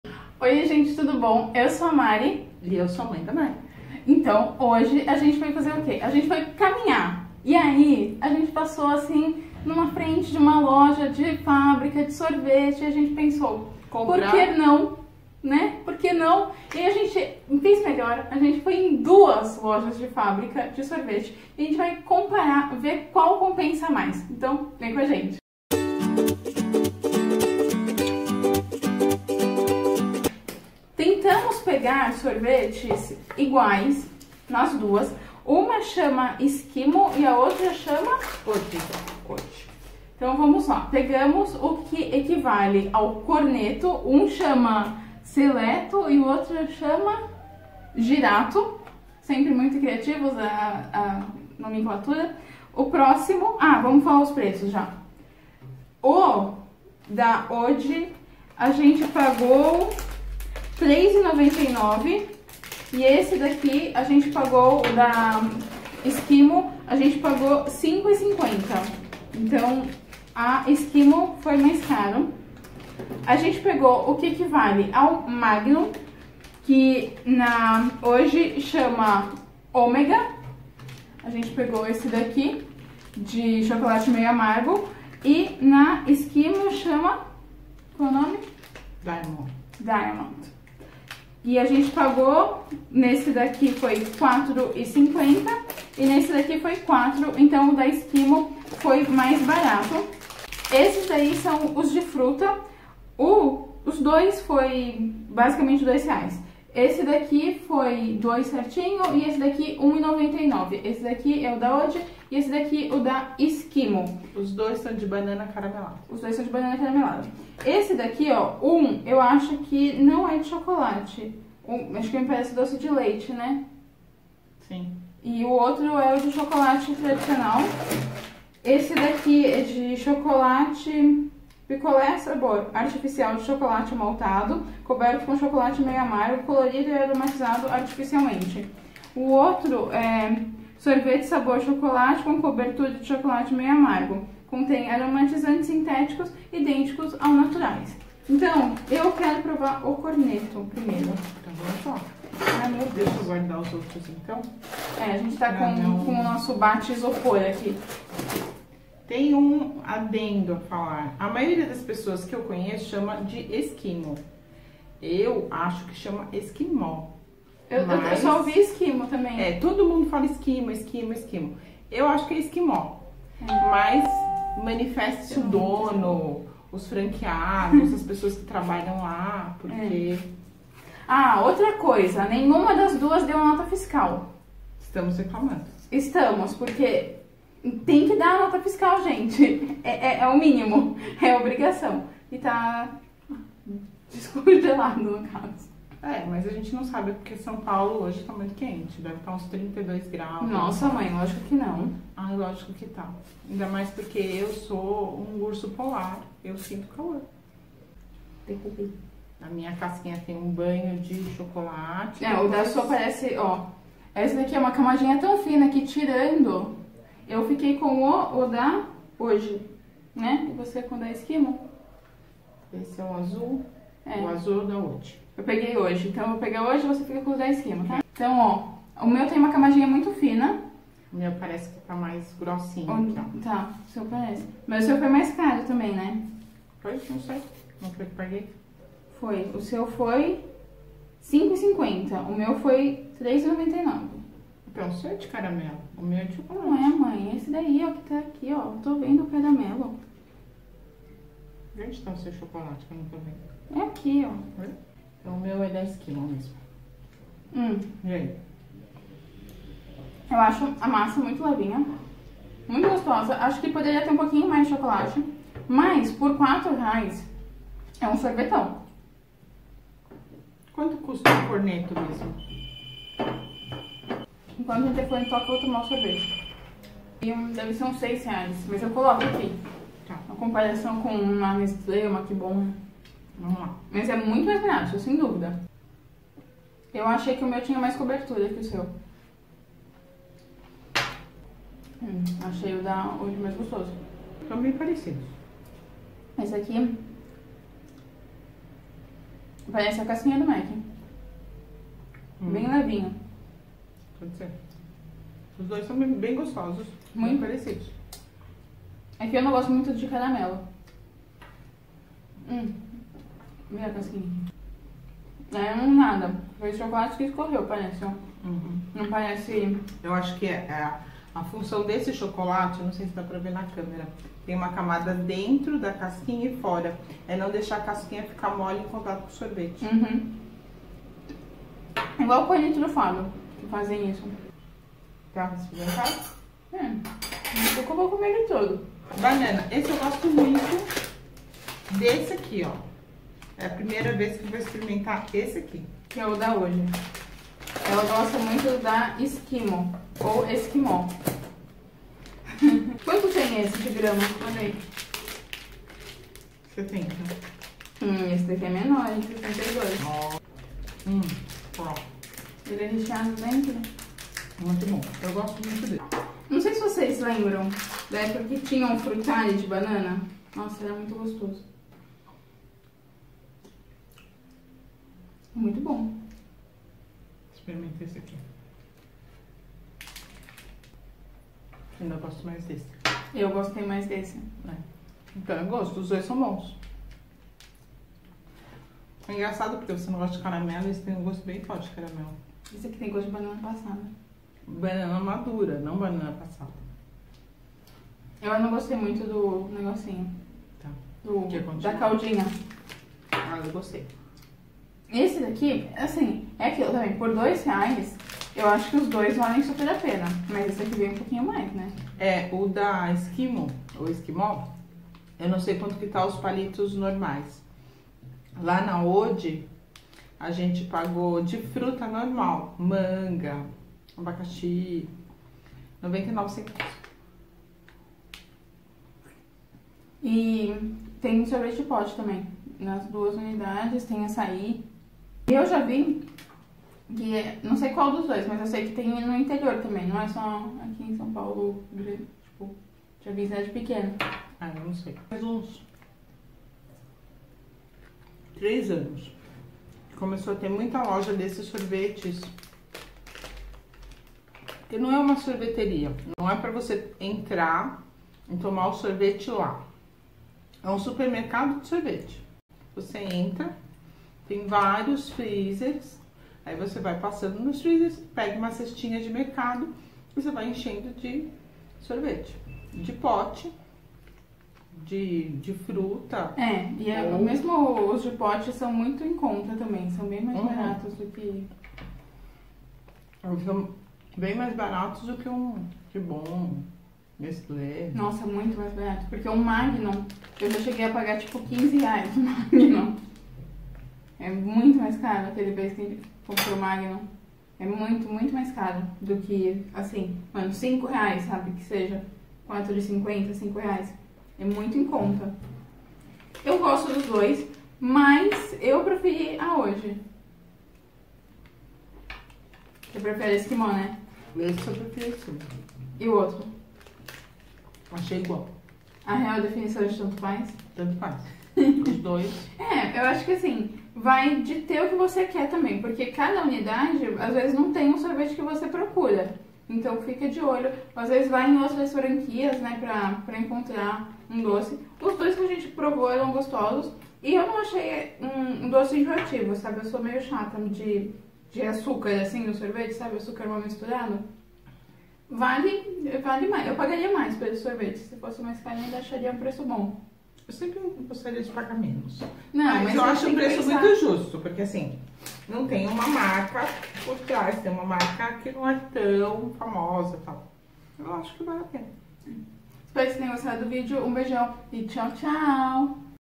Melhor LOJA de FÁBRICA de Sorvete - Oggi ou Eskimó | Mariana Oliveira
Oi oi gente, tudo bem? No vídeo de hoje eu faço uma comparação entre duas marcas de sorvete, aquelas que tem loja de fabrica, com aquele preço amigo que todo mundo gosta.

Eu comparei os preços entre a loja de fábrica da eskimó e da oggi.

um beijão e até o próximo vídeo